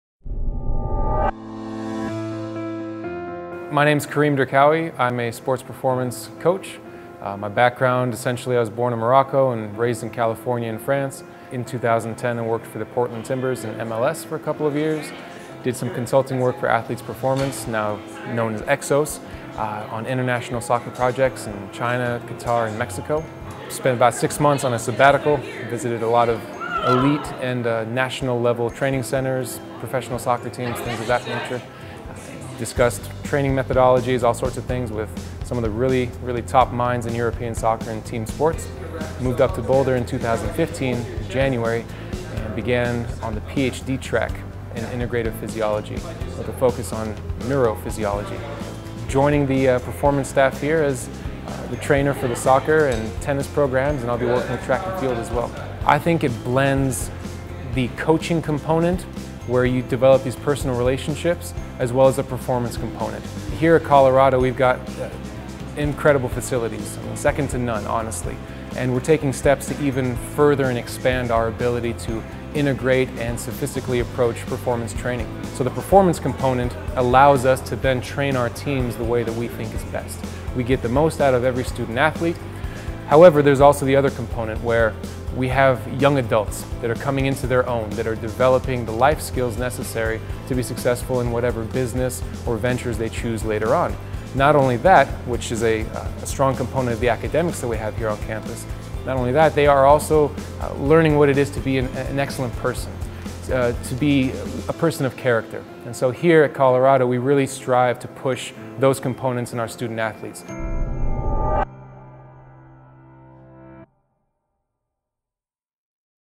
Get To Know Karim Derqaoui
New to CU, get to know Karim Derqaoui and what he brings to the Buffs as a new sports performance coach.